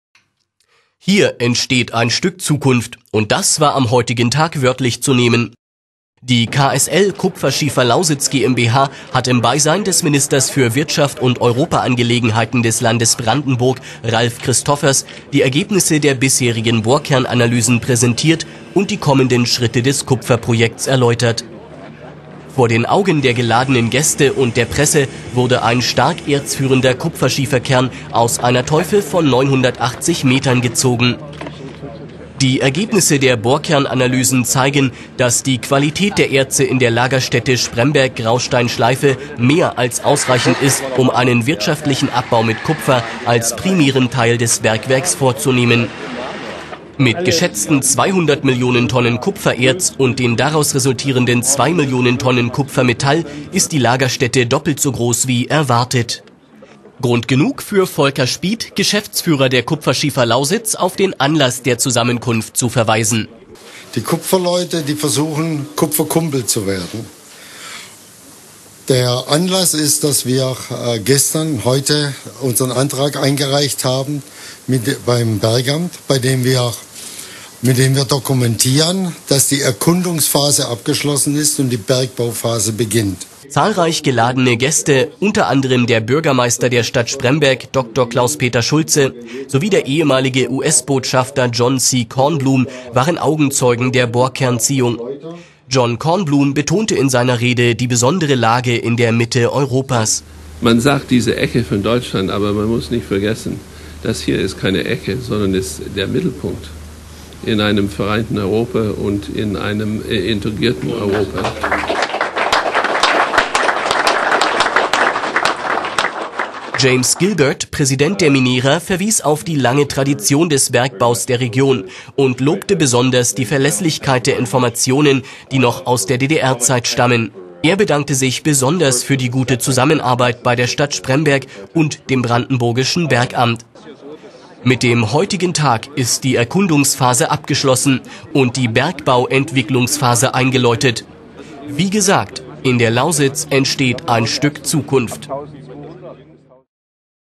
Besuch des Wirtschaftsminister Ralf Christoffers
Am Mittwoch, den 07. April 2010, hat die KSL Kupferschiefer Lausitz GmbH im Beisein des Ministers für Wirtschaft und Europaangelegenheiten des Landes Brandenburg, Ralf Christoffers, die Ergebnisse der bisherigen Bohrkernanalysen präsentiert und die kommenden Schritte des Kupfer-Projekts erläutert. Vor den Augen der geladenen Gäste und der Presse wurde ein stark erzführender Kupferschieferkern aus einer Teufe von 980 Metern gezogen. Dieser Film zeigt die Bohrkernziehung, Auszüge der Ansprachen des Geschäftsführers der KSL, Volker Spieth sowie des ehemaligen US-Botschafters John C. Kornblum und die schönsten Bilder der Veranstaltung.